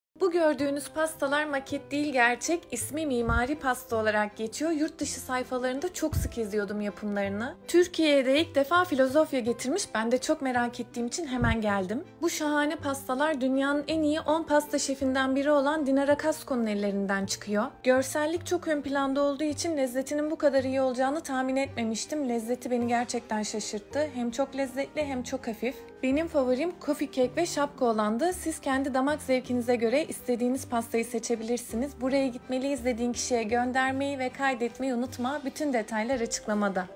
TÜRKİYEDE İLK MİMARİ YAPILARIN PASTASININ YAPAN MEKAN
💫Burası Nişantaşı’nda bulunan @flosophia_pastry flosophia_pastry Türkiye’nin ilk mimari pastalarını yapan yer. Dünyanın en iyi 10 şefinden biri olan Dinara Kasko ellerinden çıkıyo.

💫Yurt dışı sayfalarında bu pastaların yapımını çok sık izlerdim tadını da hep merak ederdim. Görsellik fazlasıyla ön planda olduğu için lezzetinin bu kadar iyi olacağını tahmin etmemiştim. Lezzeti beni şaşırttı. Pastalar hem hafif hem lezzetli.

💫Favorim Coffee Cake ve Şapka olan :) siz damak zevkinize göre istediğiniz pastayı seçebilirsiniz.

📍Açık adres: Harbiye Mahallesi Abdi İpekçi Caddesi no 57 B/13 Şişli/İstanbul

📍30A veya 30M numaralı otobüs hatlarını kullanarak veya Osmanbey Metro Durağı’nda inip Pangaltı Çıkışından çıkarak 7-8 dk yürüdüğünüzde buraya ulaşabilirsiniz.

💰Tek kişilik pasta çeşitleri 220₺